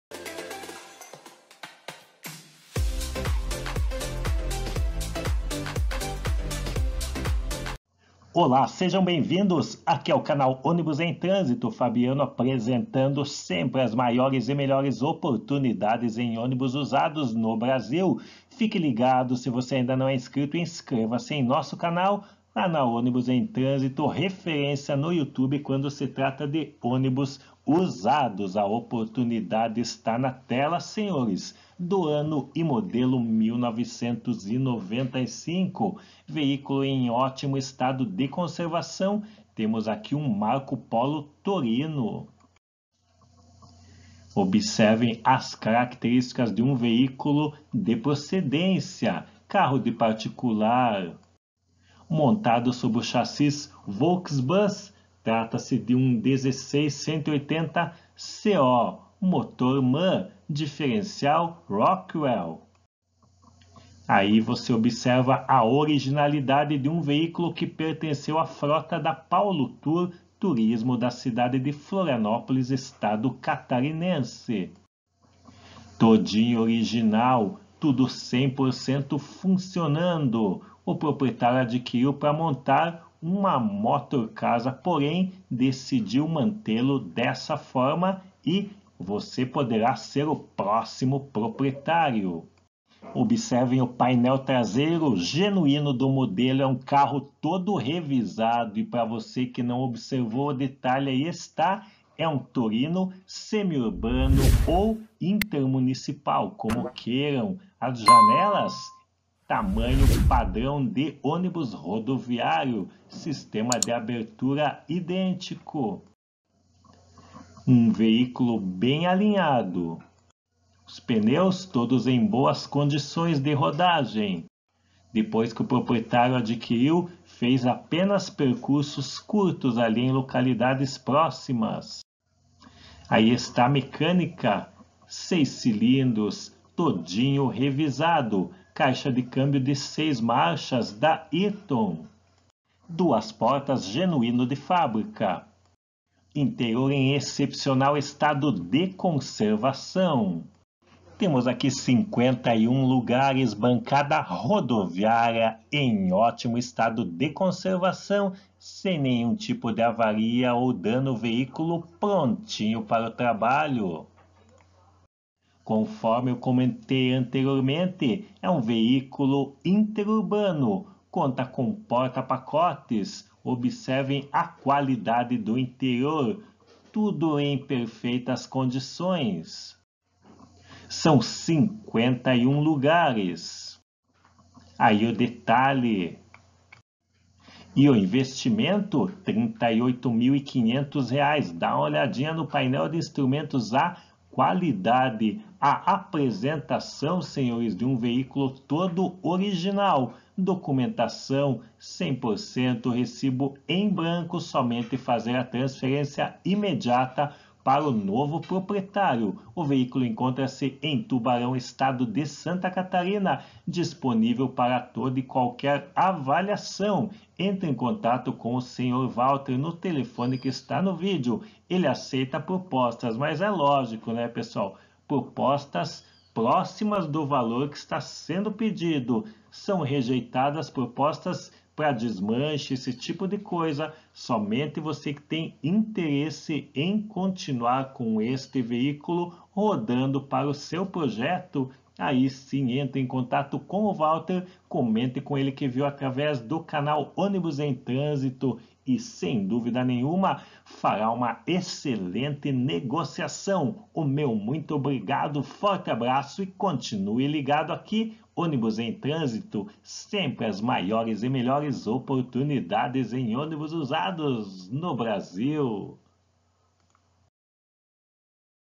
MARCOPOLO TORINO RODOVIÁRIO, VENDIDO👆🏼😉
UM RARO EXEMPLAR À VENDA, EX PAULOTOUR !  MARCOPOLO TORINO GV ANO/MOD 1995 - VOLKBSBUS 16-180 CO